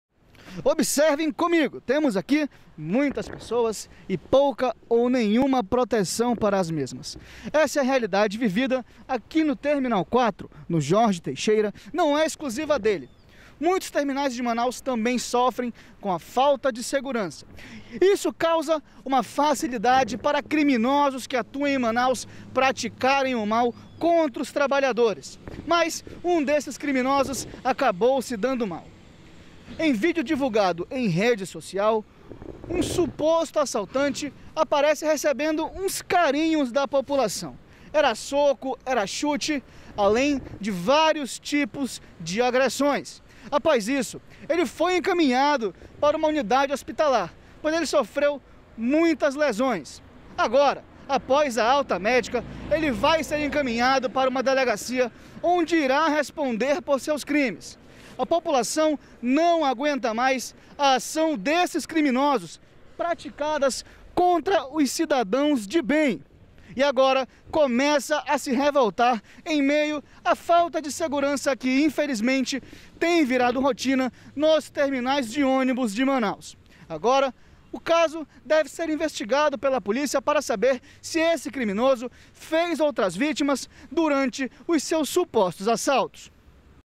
Foi roubar e pegou `peia`
Homem apanha em terminal de onibus após tentar assaltar local